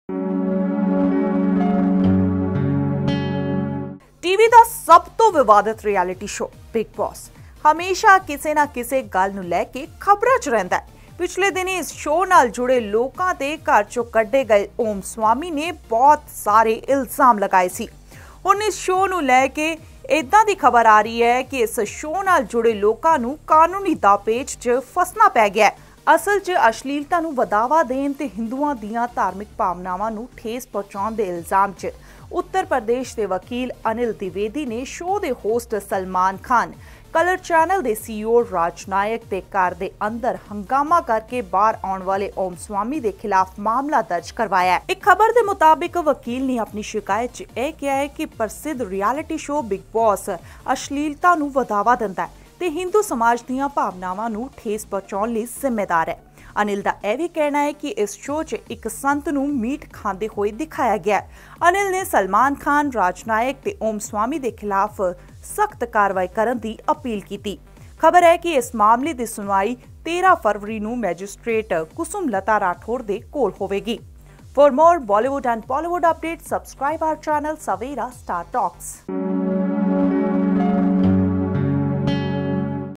Case against Salman Khan, Bigg Boss contestant Swami Om for promoting obscenity |Dainik Savera
Bigg Boss 10 Controversies